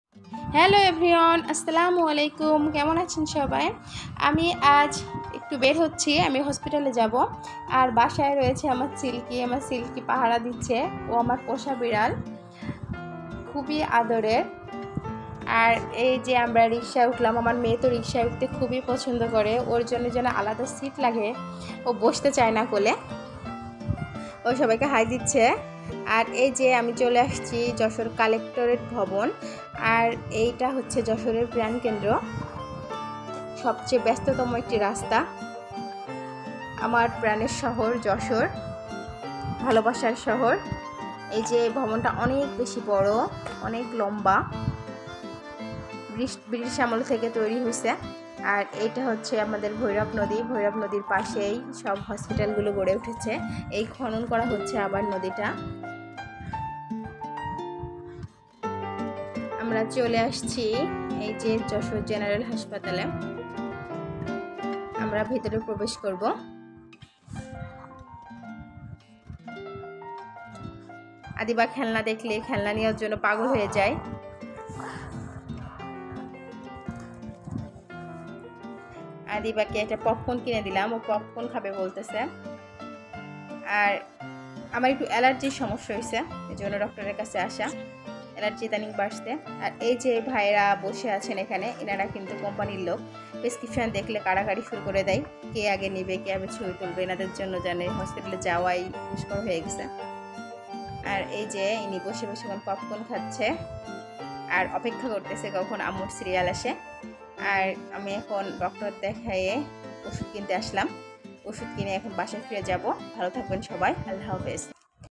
হঠাৎ হাসপাতালে যেতে হচ্ছে 😱vlog
আমার অনেক এলার্জি হয়েছে তাই হাসপাতালে যাচ্ছি। আমি আদিবা এবং আম্মু। এ খানকে ডক্টর গুলো অনেক ভালো। যশোর মানেই একটা ভালোবাসার জায়গা